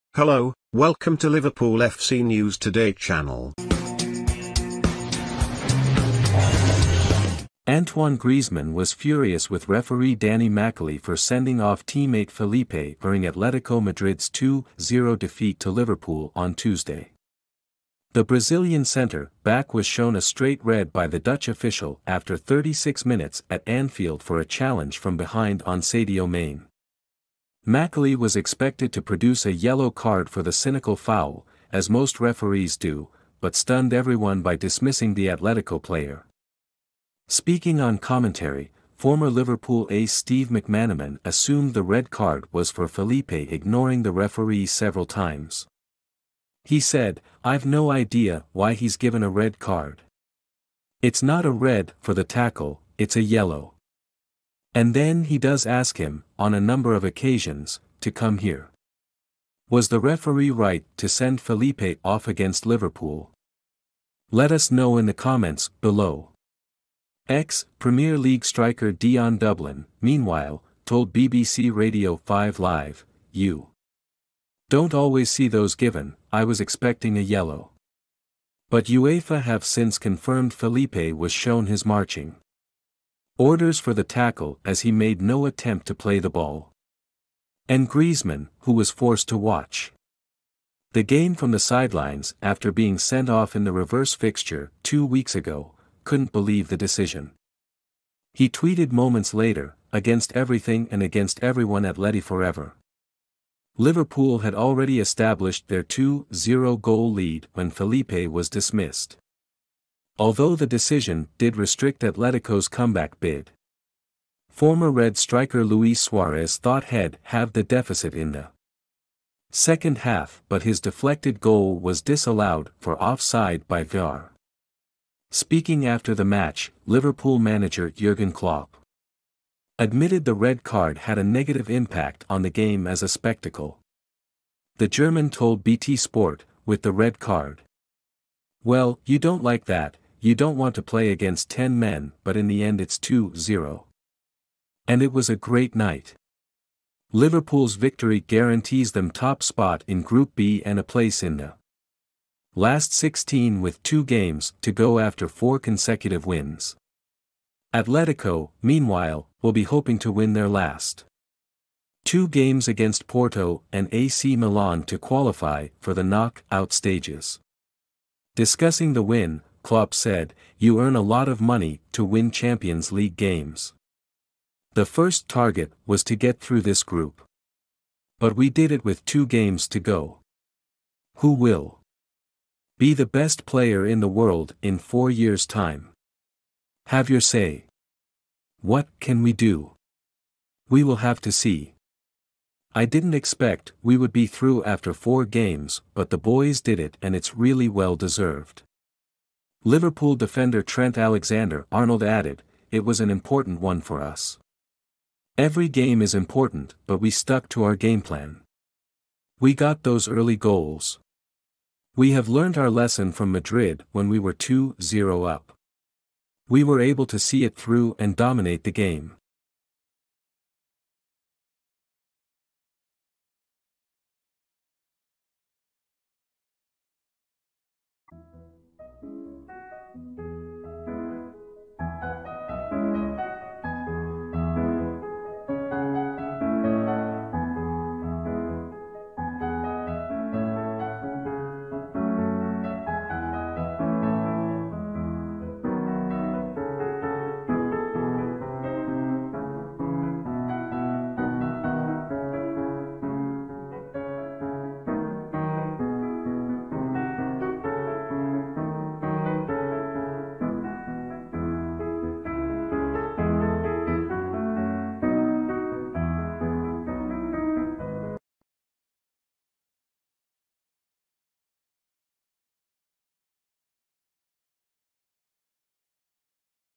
Antoine Griezmann's angry reaction to Atletico Madrid red card vs Liverpool - news today
Antoine Griezmann was furious with referee Danny Makkelie for sending off teammate Felipe during Atletico Madrid's 2-0 defeat to Liverpool on Tuesday. The Brazilian centre-back was shown a straight red by the Dutch official after 36 minutes at Anfield for a challenge from behind on Sadio Mane. M...